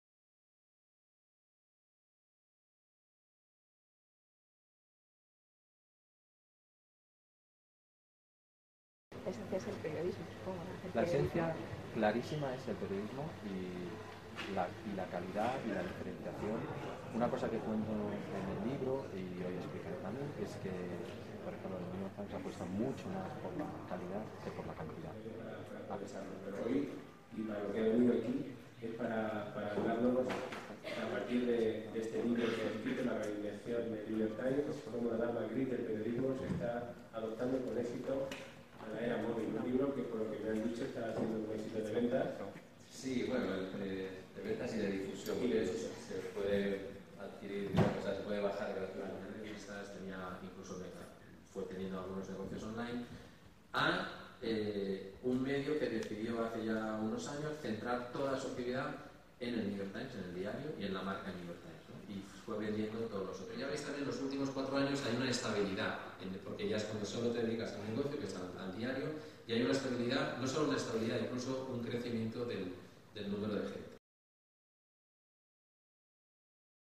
Conferencia Ismael Nafría 360º
La conferencia inaugural del Curso 2017/2018 en la Facultad de Periodismo de Cuenca corrió a cargo de Ismael Nafría, periodista que ha investigado la reinvención de The New York Times. En la mesa se habló de innovación, modelos de negocio y calidad periodística.